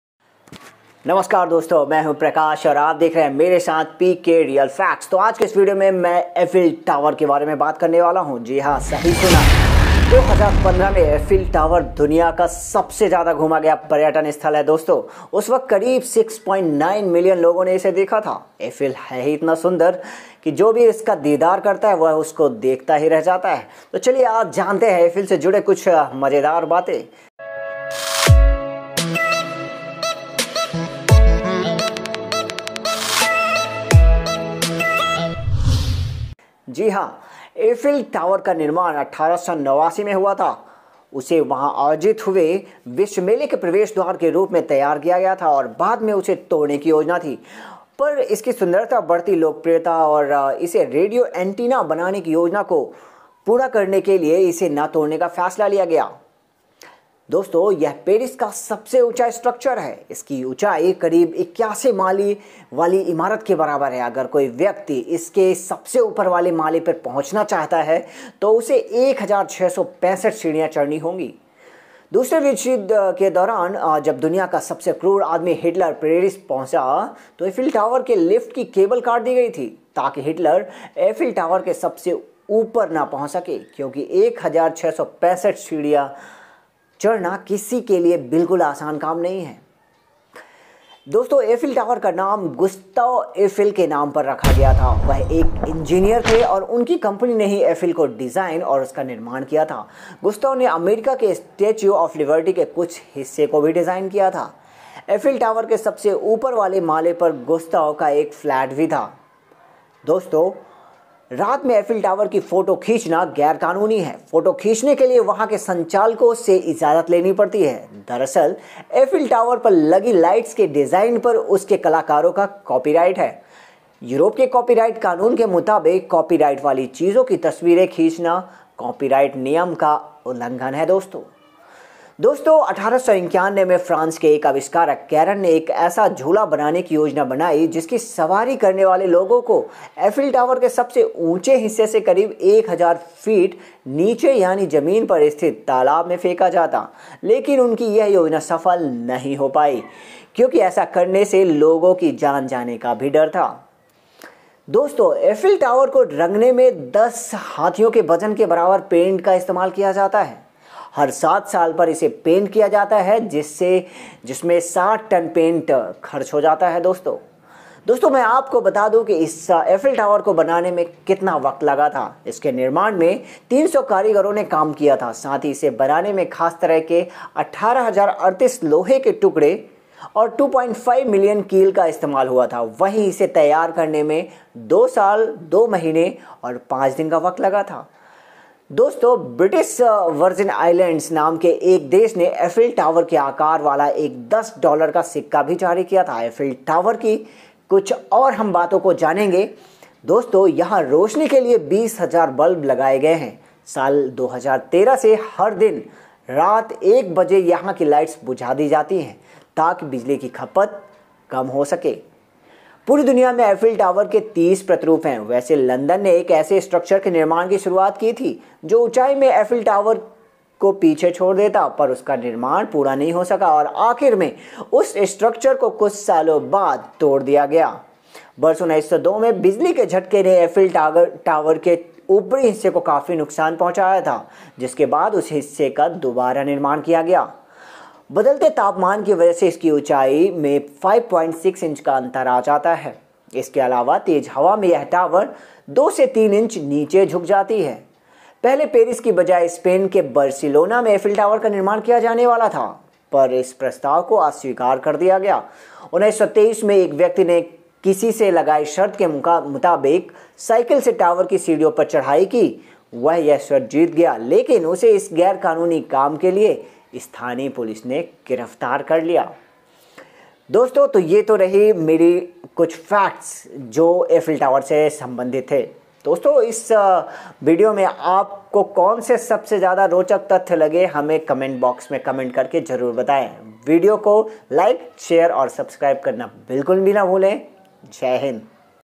Amazing Real Facts of Eiffel Tower in Hindi | Eiffel Tower का रोचक इतिहास | PkRealFacts
Effil Tower में कितने माले के बराबर ऊंचाई है
Eiffel tower की ऊंचाई कितनी है